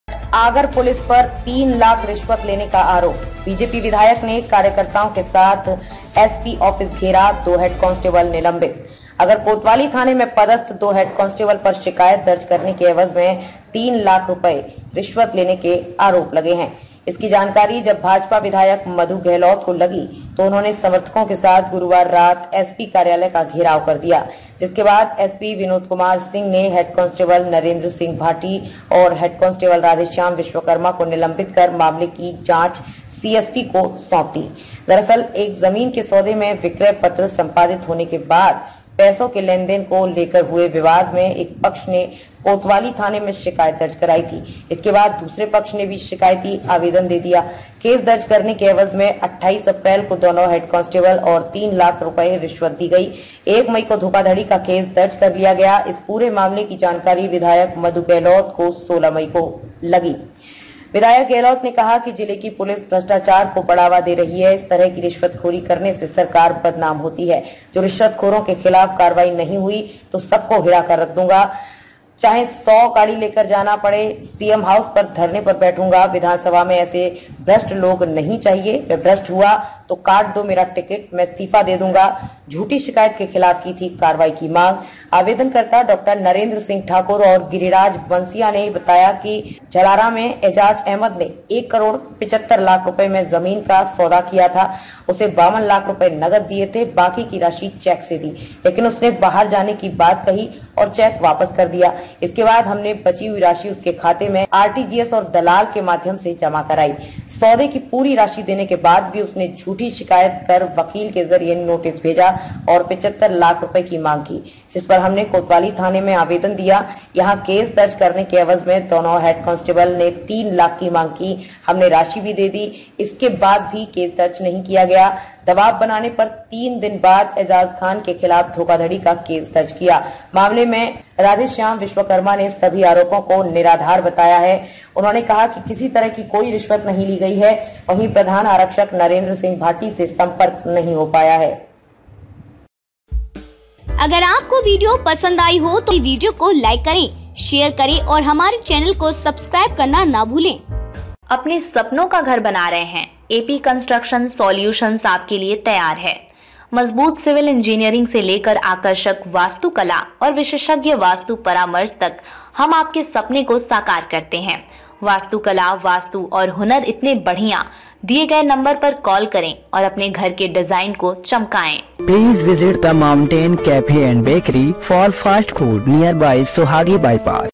Agar Police पर ₹3 लाख रिश्वत लेने का आरोप, दो head constables suspended
आगर कोतवाली थाने में पदस्थ दो हेड कॉन्स्टेबल पर शिकायत दर्ज करने के एवज में 3 लाख रुपए रिश्वत लेने के आरोप लगे हैं। इसकी जानकारी जब भाजपा विधायक मधु गेहलोत को लगी तो उन्होंने समर्थकों के साथ गुरुवार रात एसपी कार्यालय का घेराव कर दिया। जिसके बाद एसपी विनोद कुमार सिंह ने हेड कॉन्स्टेबल नरेंद्र सिंह भाटी और हेड कॉन्स्टेबल राधेश्याम विश्वकर्मा को निलंबित कर मामले की जांच सीएसपी को सौंप दी।